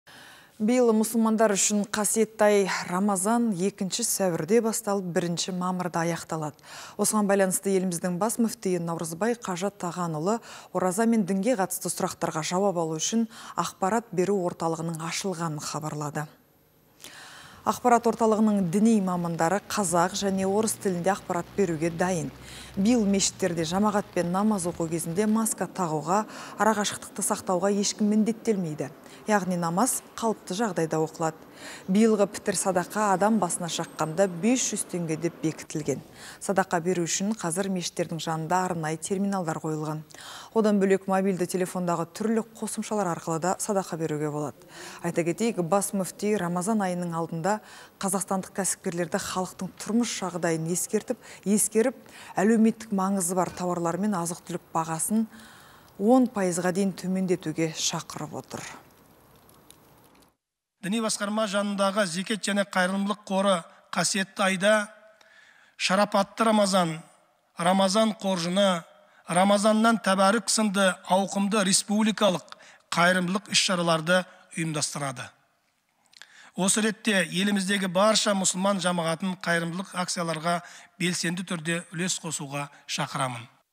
Ниет қабыл болсын! Қасиетті Рамазан айы екінші сәуірде басталады
Биыл мұсылмандар үшін қасиетті ай Рамазан екінші сәуірде басталып, бірінші мамырда аяқталады. Осыған байланысты еліміздің бас мүфтиі Наурызбай қажы Тағанұлы ораза мен дінге қатысты сұрақтарға жауап алу үшін ақпарат беру орталығының ашылғанын хабарлады.
Ақпарат орталығының діни мамандары қазақ және орыс тілінде ақпарат беруге дайын. Биыл мешіттерде жамағатпен намаз оқу кезінде маска тағуға, ара қашықтықты сақтауға ешкім міндеттелмейді. Яғни, намаз қалыпты жағдайда оқылады. Биылғы пітір-садақа адам басына шаққанда 500 теңге деп бекітілген. Садақа беру үшін қазір мешіттердің жанында арнайы терминалдар қойылған. Одан бөлек, мобильді телефондағы түрлі қосымшалар арқылы да садақа беруге болады. Айта кетейік, Бас мүфти рамазан айының алдында қазақстандық кәсіпкерлерді халықтың тұрмыс жағдайын ескеріп, әлеуметтік маңызы бар тауарлар мен азық-түлік бағасын10 пайызға дейін төмендетуге шақырып отыр.
НАУРЫЗБАЙ ҚАЖЫ ТАҒАНҰЛЫ, Қазақстан мұсылмандары діни басқармасының төрағасы, Бас мүфти:
– Діни басқарма жанындағы Зекет және қайырымдылық қоры қасиетті айда "Шарапатты Рамазан", "Рамазан қоржыны", "Рамазаннан тәбәрік" сынды ауқымды республикалық қайырымдылық іс-шараларды ұйымдастырады. Осы ретті еліміздегі барша мұсылман жамағатын қайырымдылық акцияларға белсенді түрде үлес қосуға шақырамын.